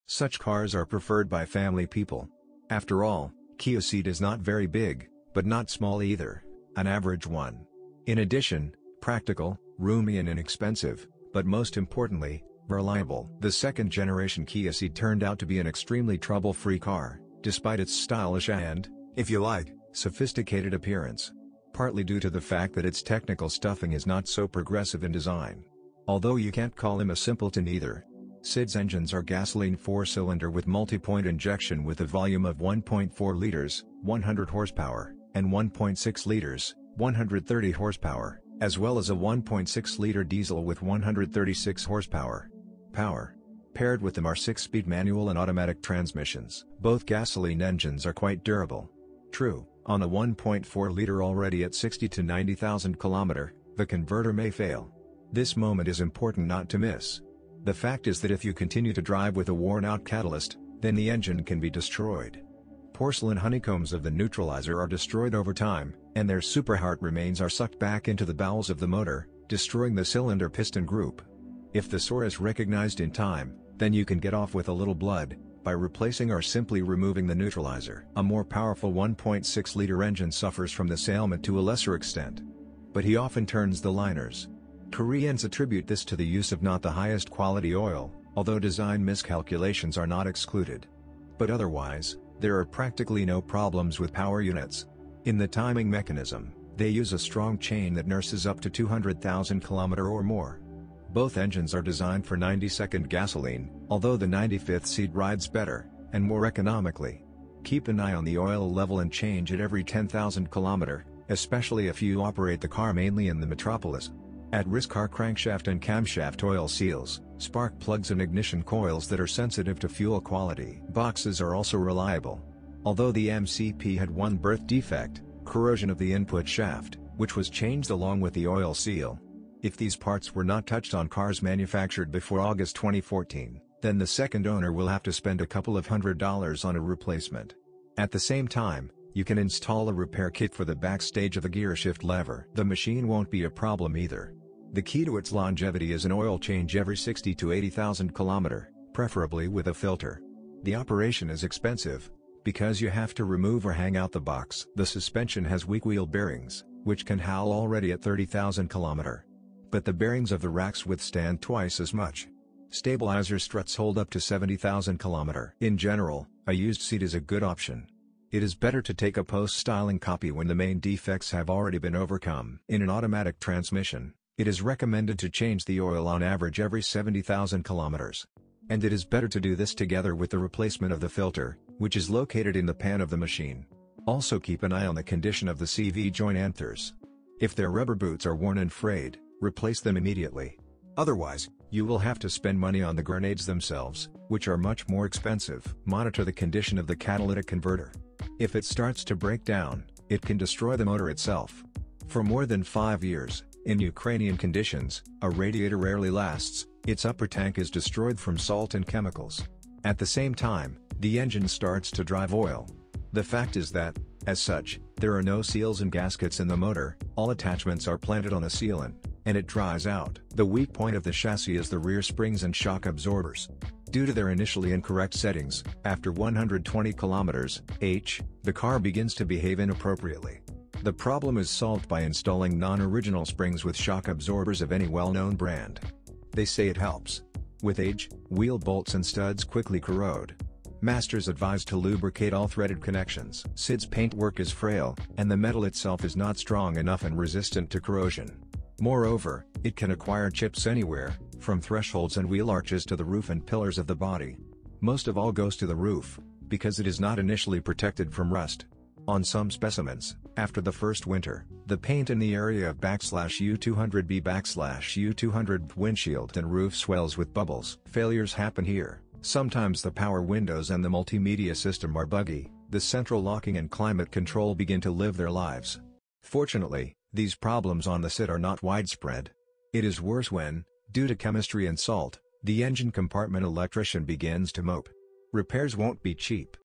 Kia Ceed 2012 - 2018 - high Miles review
Common Issues Kia Ceed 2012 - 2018 generation